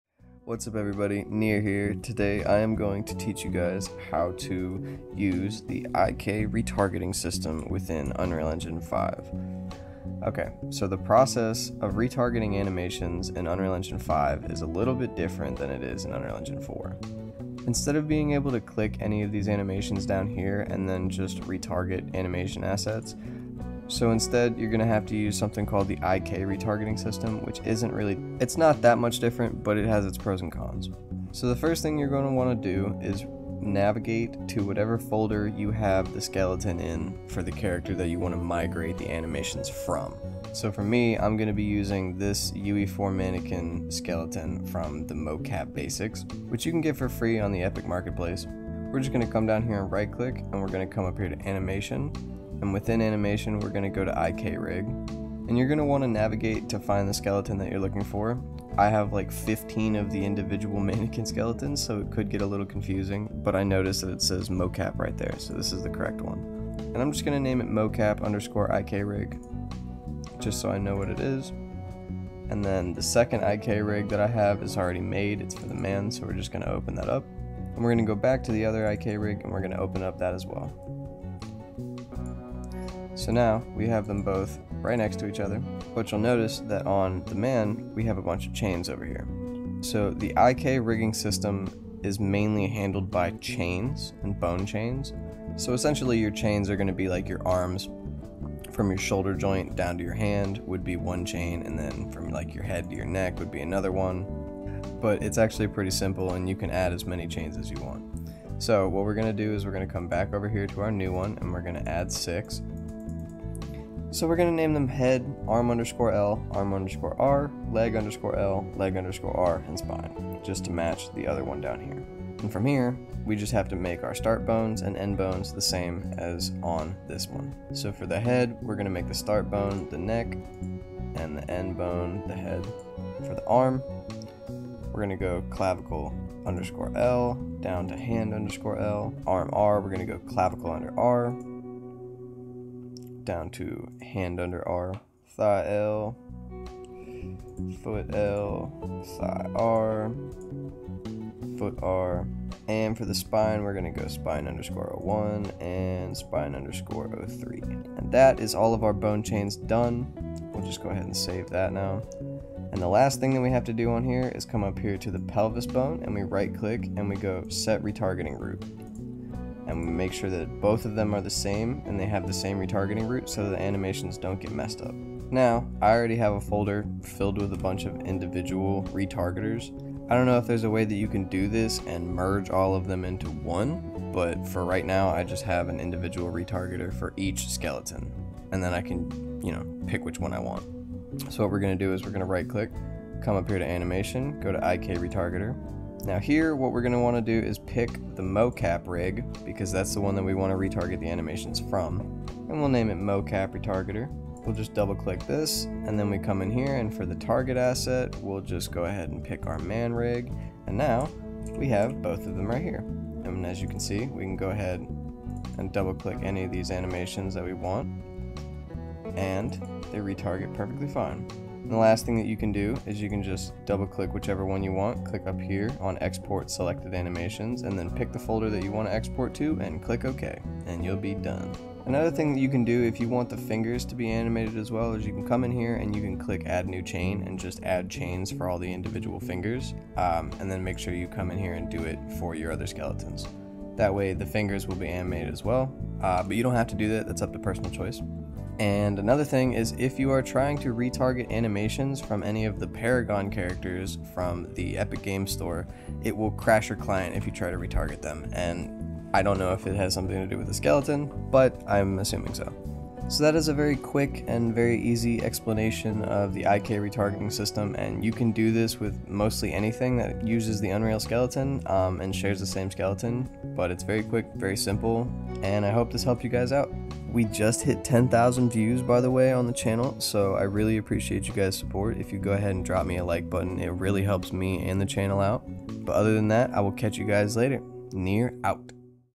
Unreal Engine 5 Tutorial: Animation Retargeting/IK Retargeting System
Hey Homies! Today I'm bringing you a quick little tutorial on how to set up and use the IK Retargeting System within Unreal Engine 5.
You can take this and expand on it as much as you want!
Thank you guys for the support! We just hit 10K views on the channel and we're close to 100 Subscribers as well!
All love.